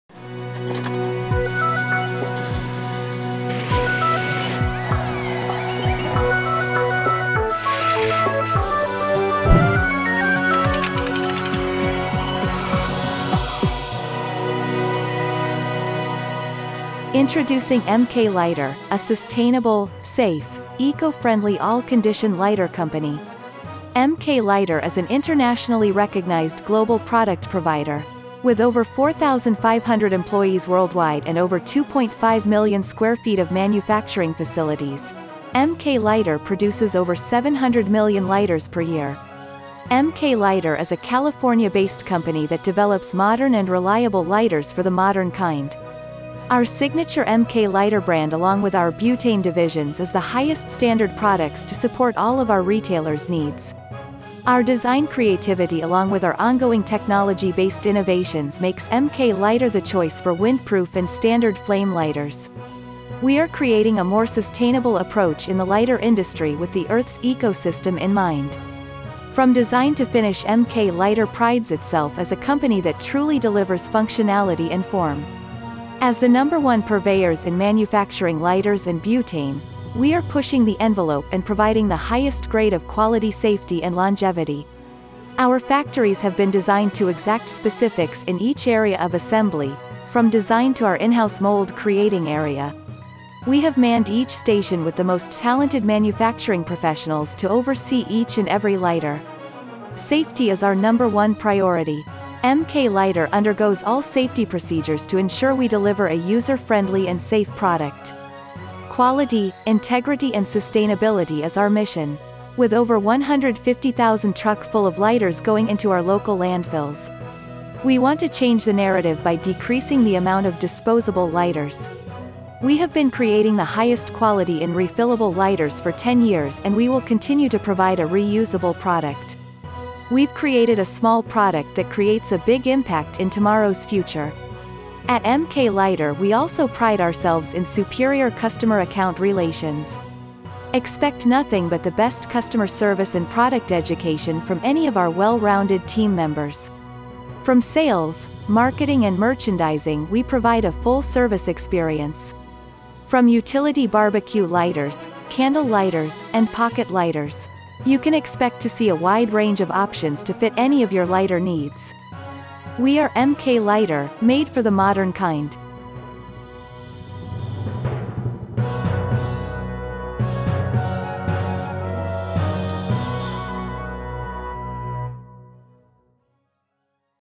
MK Lighter Company
MK Lighter is an internationally recognized Global Product Provider. With over 4,500 employees worldwide and over 2.5 million square feet of manufacturing facilities, we produce over 700 million lighters per year.